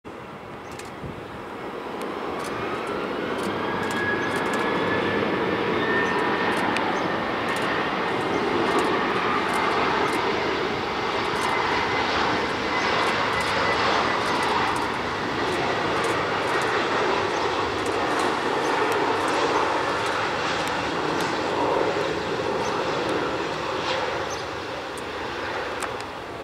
RETURN!!! MEA/ Beirut to Nice Côte D'Azur/ A21N
Immatriculation: T7-ME6
Appareil: Airbus A321-271NX
Lieu: Nice Airport/LFMN